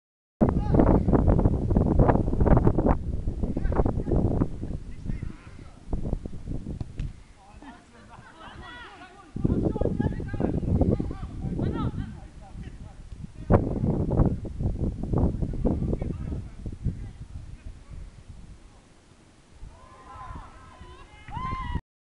St Stephen AFC Training on 3G Pitch on Tuesday 4th October 2016
Highlights. Give and go Jason Rundle passes to Reece Willmott and then runs in on goal as Reece sends in a cross which Jason heads in a double 1 - 2, now score is 3 - 1